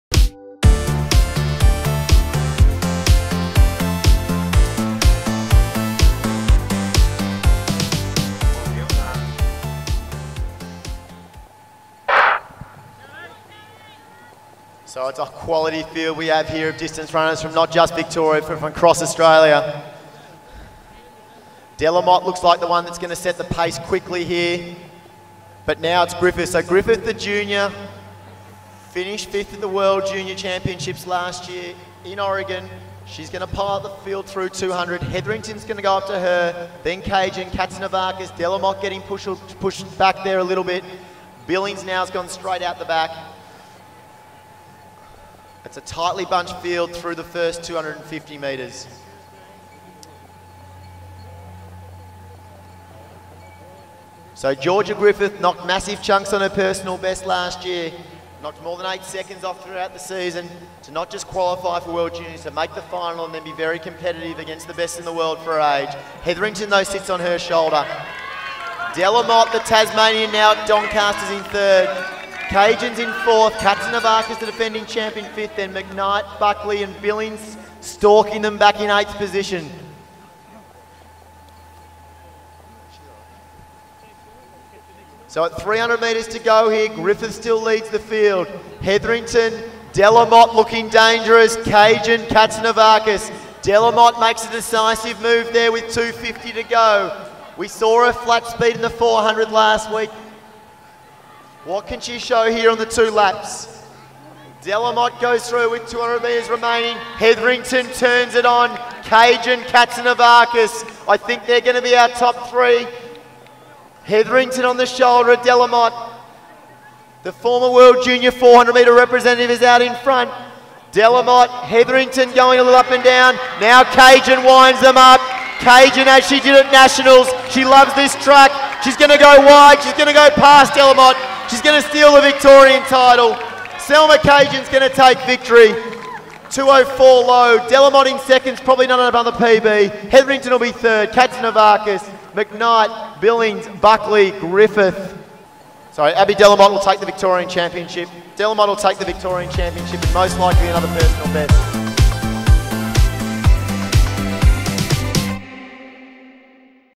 2015 Vic Open Champs, Women 800m Final
2015 Victorian Open Track & Field Championships. Held at Lakeside Stadium, 27 Feb - 1 March 2015.
Men's 800m Final.
Features Salma Kajan, Abbey de la Motte, Kelly Hetherington.